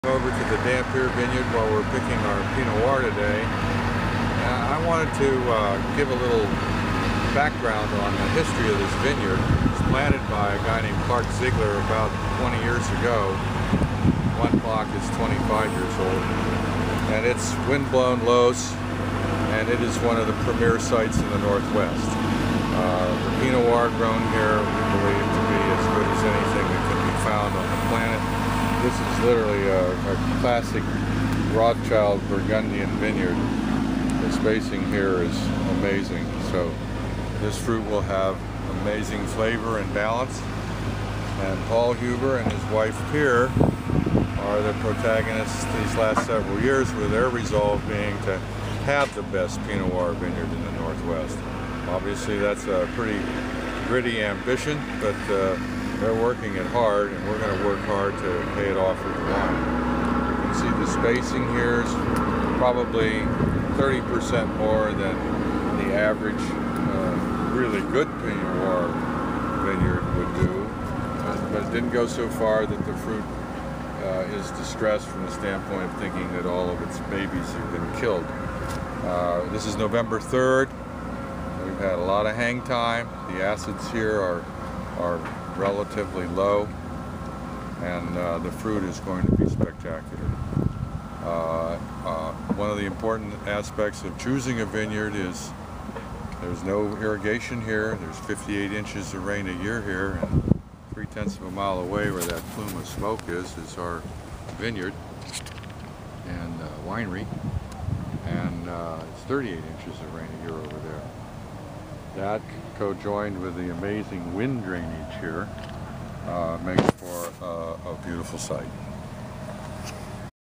Cathedral Ridge Winery Dampier Vineyard Pinot Noir 2010 Harvest
Harvest time in the Award Winning Dampier Vineyard on November 3rd 2010.
This wine went on to win "Best of Show Pinot Noir" at the NW Wine Summit 2013.
Delicious!